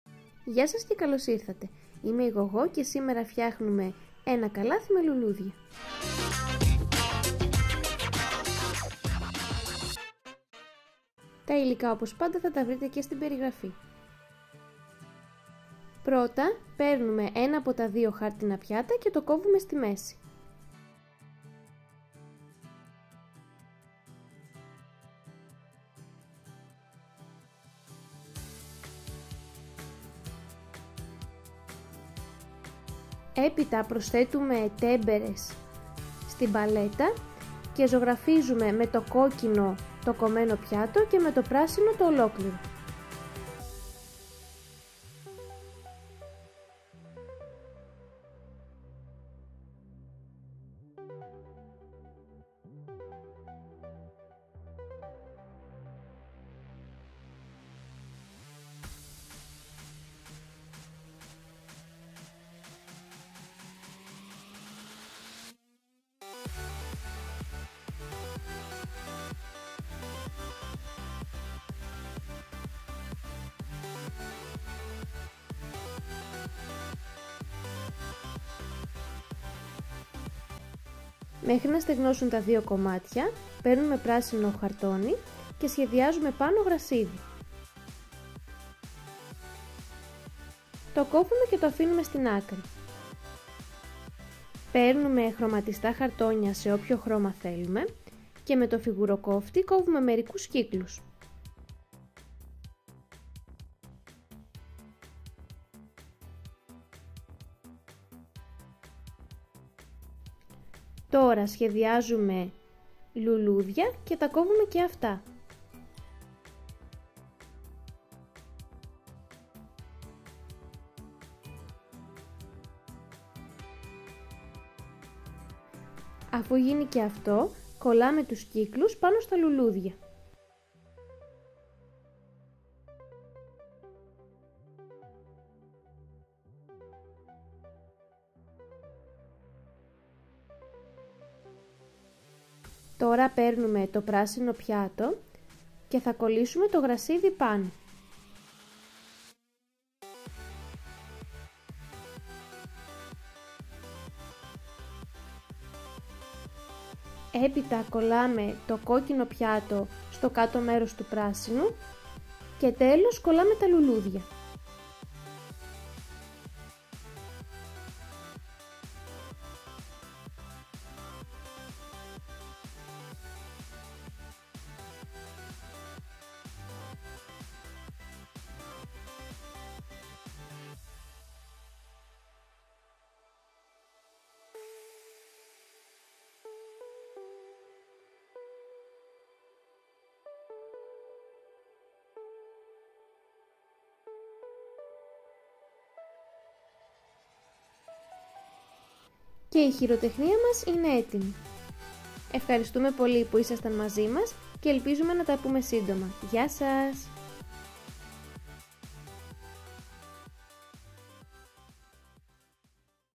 Ανοιξιάτικη χειροτεχνία: Καλάθι με λουλούδια/ Spring craft: Basket with flowers
Γεια σας και καλώς ήρθατε. Είμαι η Γωγώ και σήμερα φτιάχνουμε ένα καλάθι με λουλούδια. Τα υλικά που χρησιμοποιήσαμε είναι:
2 χάρτινα πιάτα
Ψαλίδι και κόλλα
Μολύβι και γόμα
Παλέτα και πινέλα
Κόκκινη τέμπερα
Πράσινη τέμπερα
Πράσινο χαρτόνι
Χρωματιστά χαρτόνια
Φιγουροκόφτης κύκλος
Ελπίζουμε να σας άρεσε αυτό το βίντεο και πριν φύγετε να κάνετε ένα like.

Hello and welcome. I am Georgia and today we are making a basket with flowers. The materials we used are:
2 paper plates
Scissors and glue
Pencil and eraser
Palette and brushes
Red tempera
Green tempera
Green construction paper
Colored construction paper
Perforator circle

❤️facebook/ Paint Love Create ❤️